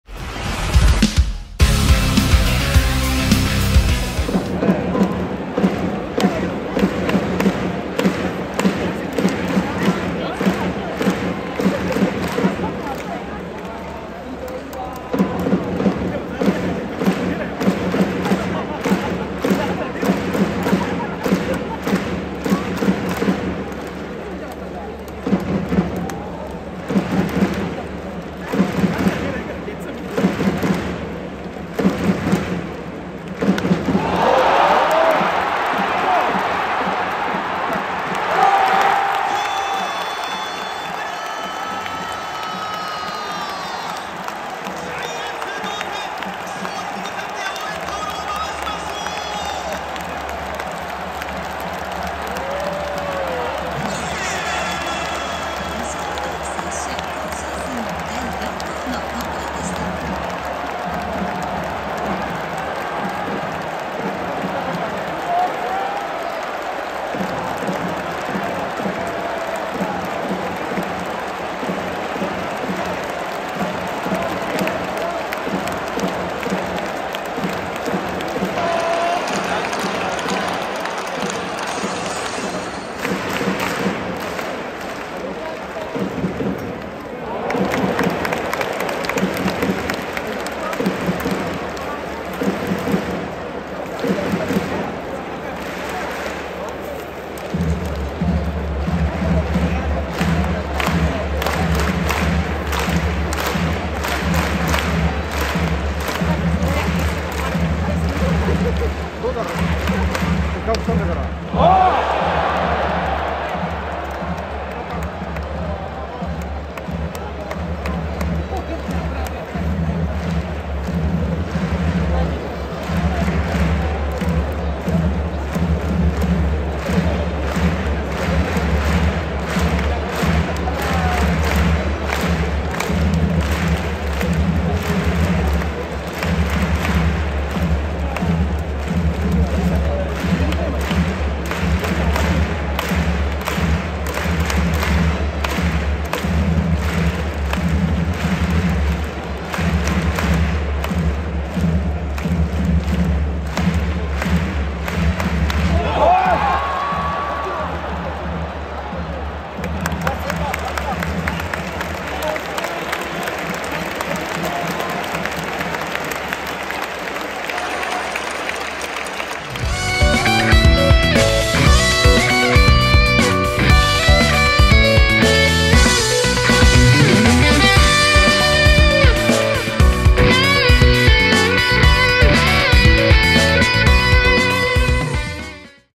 巨人吉川尚輝、9回裏劇的ホームランの瞬間！ツーアウト同点HRで延長戦突入！巨人vs横浜DeNAベイスターズ 2022年7月8日（金）
巨人吉川尚輝、9回裏2アウト劇的ホームランの瞬間！ツーアウトから爆撃HRで延長戦突入！巨人vs横浜DeNAベイスターズ　2022年7月8日（金）